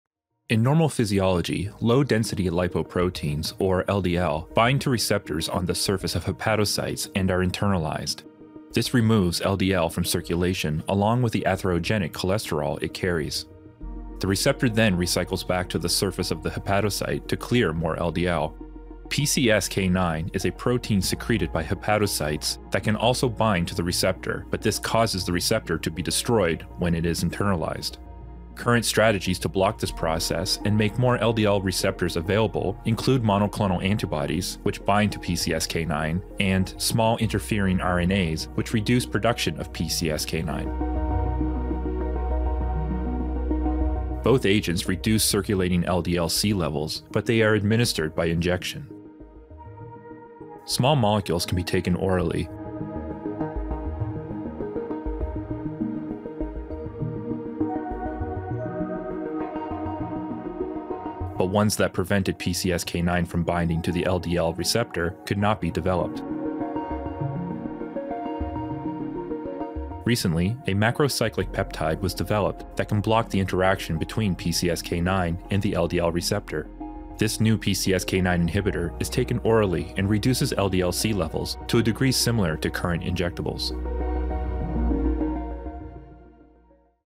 PCSK9 Inhibitors in Action
This animation details the mechanism of action of oral PCSK9 inhibitors.